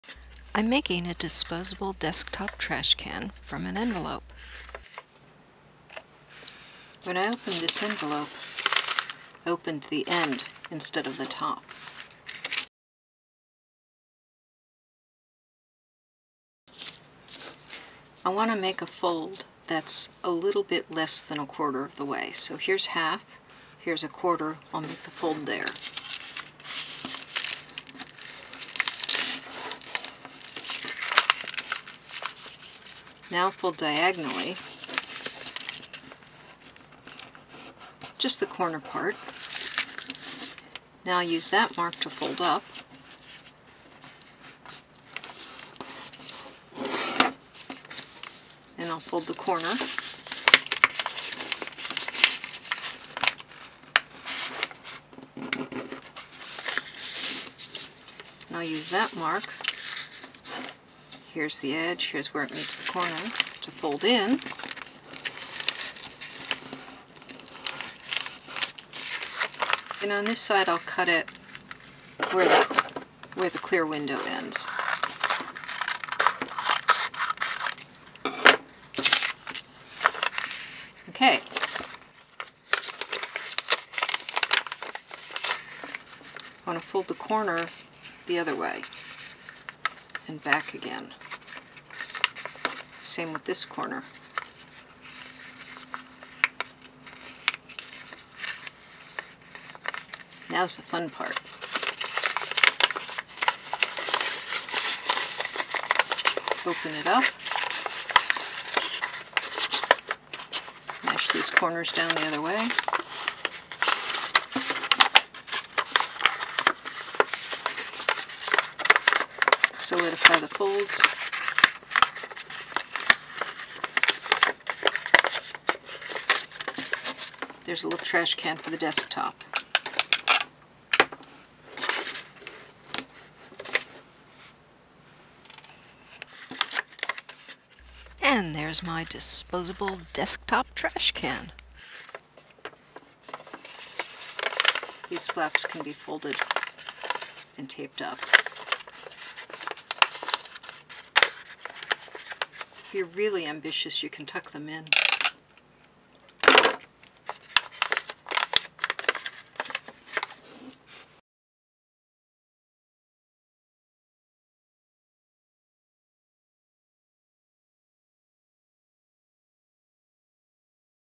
Folding a Benchtop Mini Trash Can from an Envelope (version 2—simpler!)
(Links below) I made a mini desktop trash can out of an envelope. (Again. This one's much simpler than last time!) I opened the envelope on a short end, but it also works to tape the top closed again. Or, of course, start with an unused envelope.

"Mini Trash Can From and Envelope for Workbench/Desktop (Disposable)" Instructable is

Content:
00:00 Opening title/intro
00:05 Envelope—slit end
00:18 Fold sides, corners, bottom
01:09 Cut
01:21 Corners again
01:40 Open, straighten
02:11 Tape down/end result
02:23 Optional: Tape corners
02:41 End screen